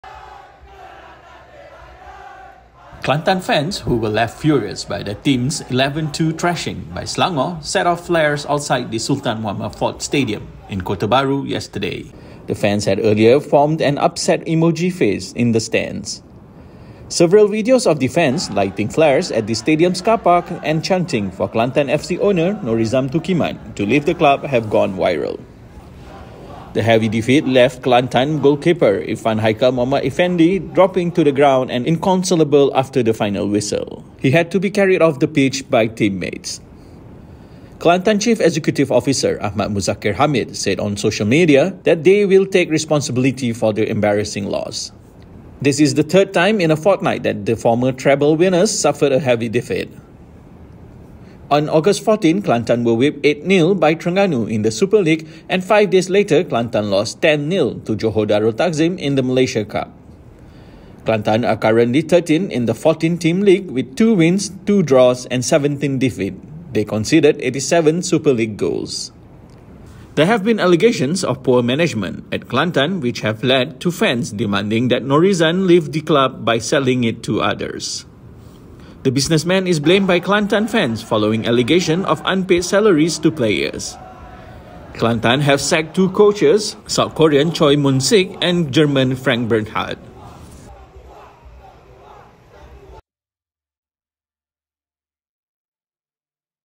Furious Kelantan fans ignite flares after 11-2 loss
Kelantan fans, who were left furious by their team’s 11-2 thrashing by Selangor, set off flares outside the Sultan Muhammad IV Stadium in Kota Baru yesterday.

The fans had earlier formed an “upset emoji face” in the stands.

Several videos of the fans lighting  flares at the stadium's car park and chanting for Kelantan FC  owner Norizam Tukiman to leave the club have gone viral.

The heavy defeat left   Kelantan goalkeeper Irfan Haikal Mohd Iffendi dropping to the ground and  inconsolable after the final whistle. He had to be carried off the pitch by teammates.

Kelantan chief executive officer Ahmad Muzakkir Hamid said on social media that they  will take responsibility for the embarrassing loss.

This is the third time in a fortnight that the former treble winners suffered a heavy defeat.

On Aug 14, Kelantan were whipped 8-0 by Terengganu in the Super League and five days later, Kelantan lost 10-0 to Johor Darul Ta'zim in the  Malaysia Cup.

Kelantan are currently 13th in the 14-team league with two wins, two draws and 17 defeats. They conceded 87 Super League goals.

There have been allegations of poor management at Kelantan,  which have led to fans demanding that Norizam leave the club by selling it to others.

The businessman is blamed by Kelantan fans following allegations of unpaid salaries to players. Kelantan have sacked two coaches, South Korean Choi Moon Sik and German Frank Bernhardt.